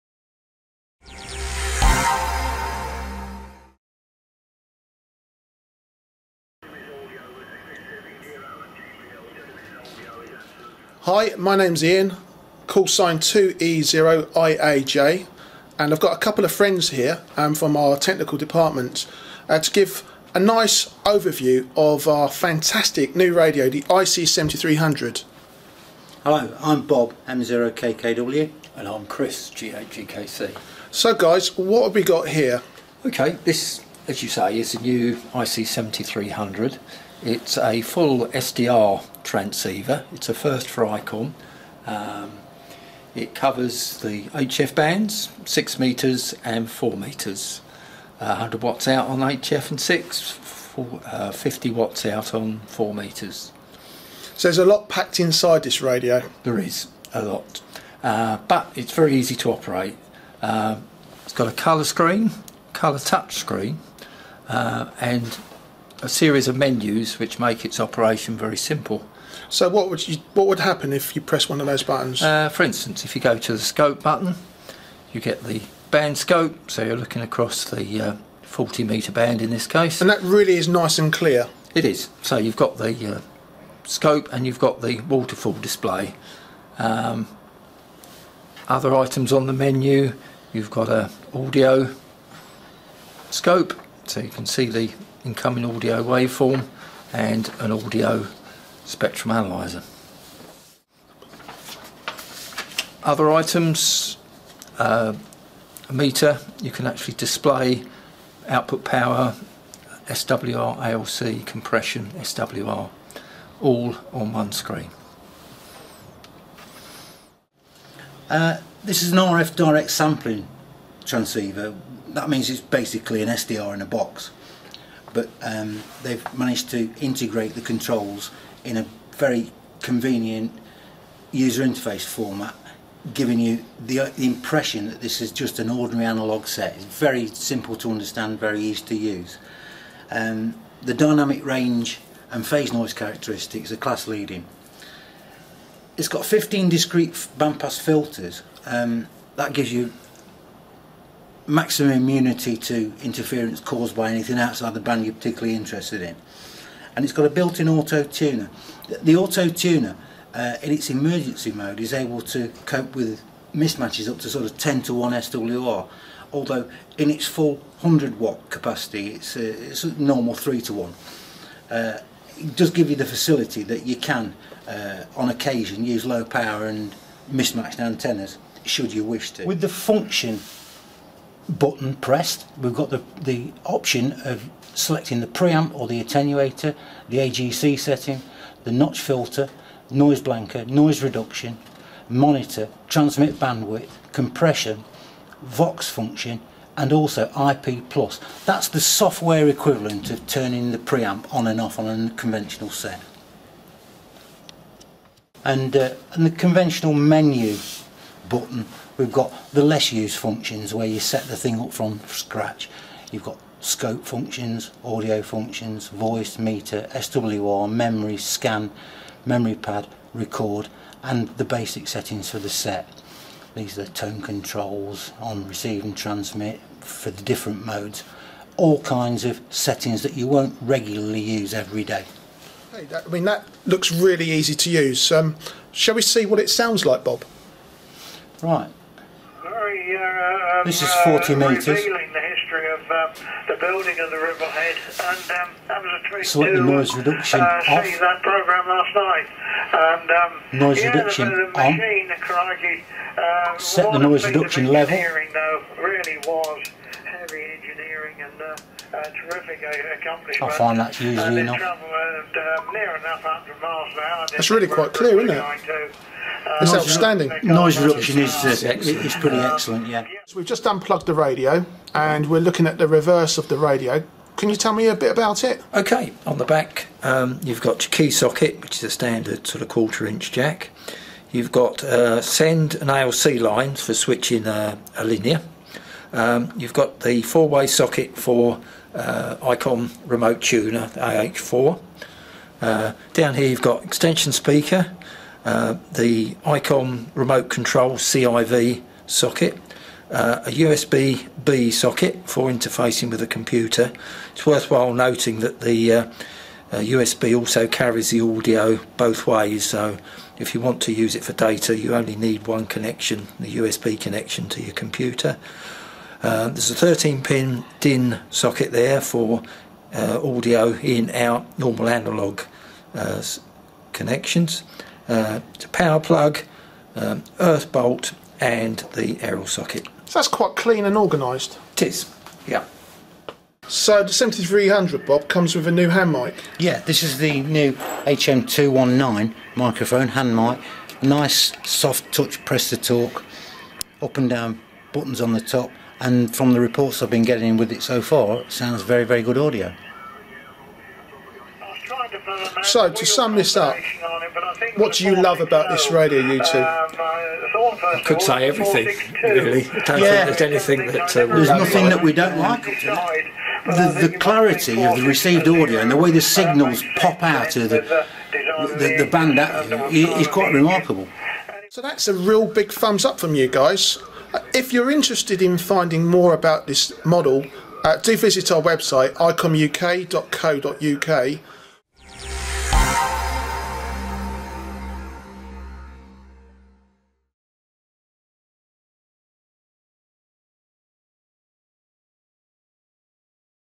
Introduction to the Icom IC-7300 HF/50/70MHz Transceiver
Chris Ridley (G8GKC), Bob Rogers (M0KKW) and Ian Lockyer (2E0IAJ) from Icom UK give an overview of the IC-7300 HF/50/70MHz transceiver, Icom's first Software Defined Radio (SDR) HF radio.

In this video the Icom team give an overview of its many functions/features that include:

• High Quality Real-Time Spectrum Scope
• RF Direct Sampling System
• Class Leading Phase Noise Characteristics
• High-Resolution Waterfall Function
• Audio Scope Function
• Large Touch Screen Colour TFT LCD and menu driven system
• Built-in Automatic Antenna Tuner
• Easy to hear sound quality